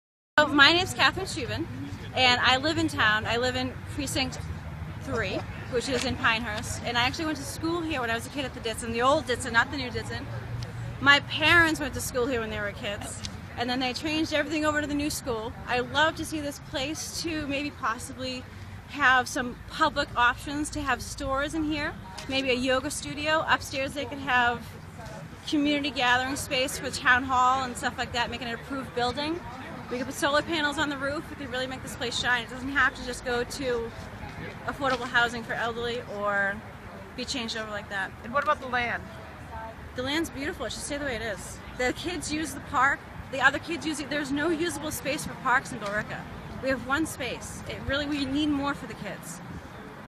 Pinehurst resident hopes reuse of old Ditson School in Billerica will benefit whole community.
Pinehurst resident hopes reuse of old Ditson School in Billerica will benefit whole community. @Lowellsunnews